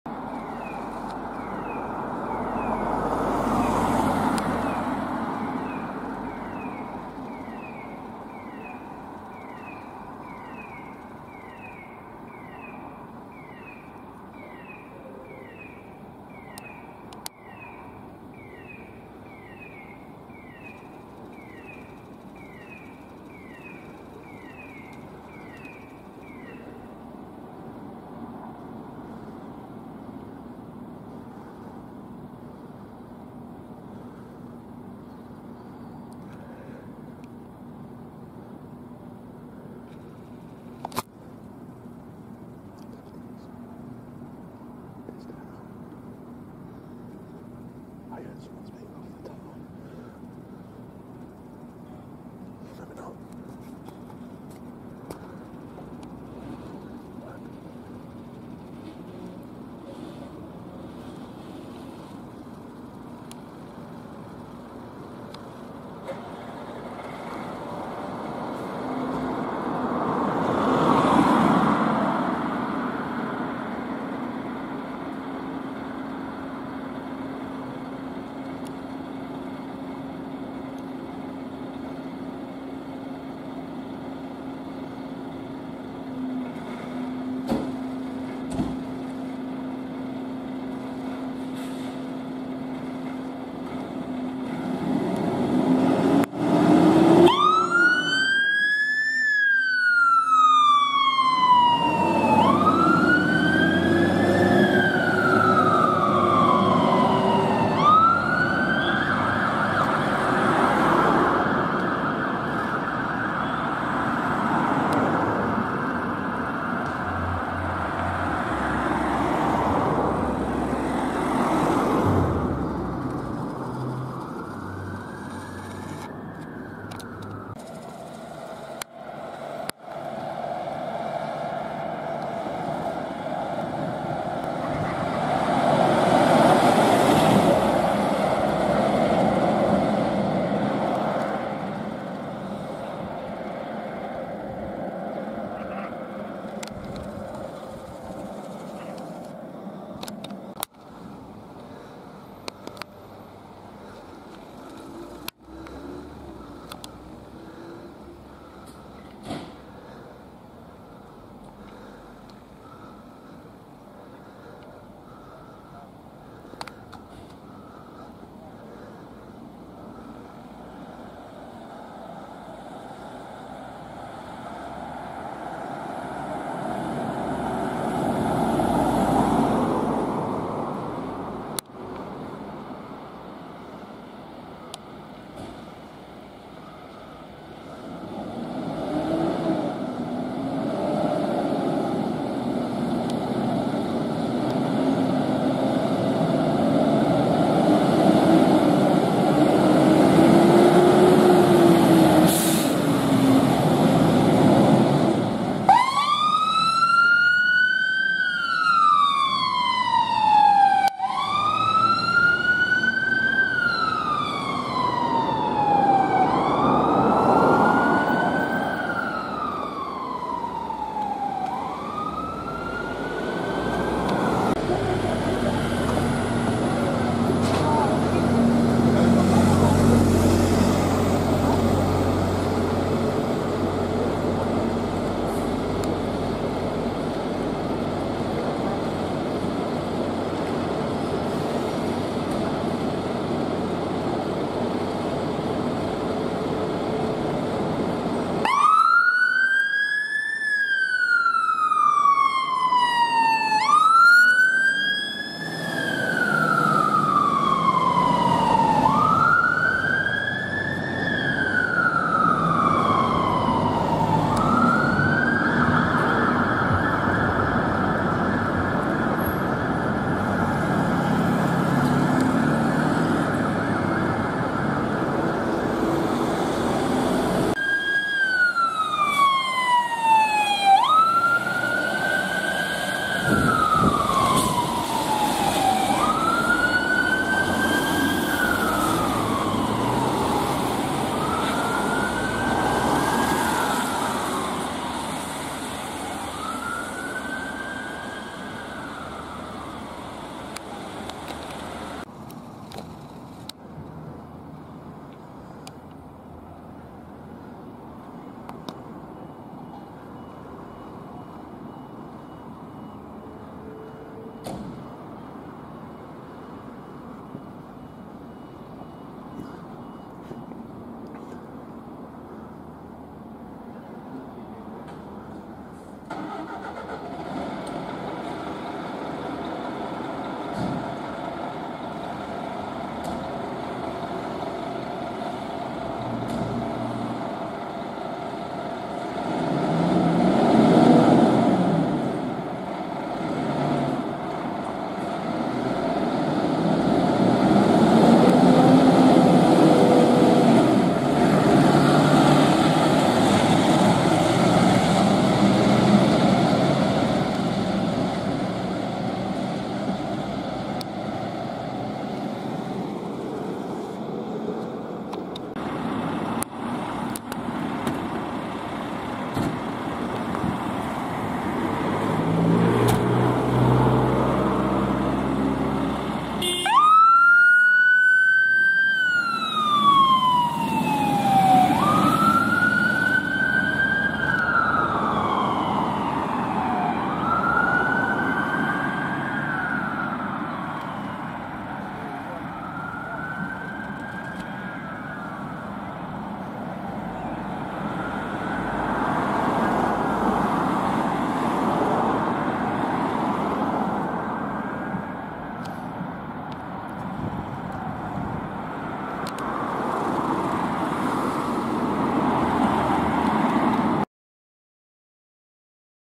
LOTS OF FIRE ENGINES RESPONDING! Various UK Fire Appliances Responding on Blue Lights and Sirens!
Fire Appliances from multiple Fire and Rescue Services from across the UK responding on blue lights and sirens!

0:00 GMFRS Manchester Central Volvo FL Reserve Pump G16P1 Turning Out to a House Fire in Moss Side
2:08 Cumbria FRS Wigton Volvo FL Pump C11P1 Responding to a Rescue At Height in Caldbeck
2:32 Cumbria FRS Kendal Volvo FL Enhanced Rescue Pump C60P1 Turning out to a Domestic Smoke Alarm Activation
3:46 LFB Soho Mercedes Atego Pump Ladder A241 turning out to a High Rise in Camden
4:37 GMFRS Whitehill Volvo FE Turntable Ladder G20A3 responding to a Rescue at Height in Offerton
5:03 Bedfordshire FRS Luton Scania P280 FGB08P2 Turning out to a Car Fire in Luton
6:07 Cumbria FRS Carlisle East Volvo FL Pump C20P1 Turning out to a Animal Rescue in Carlisle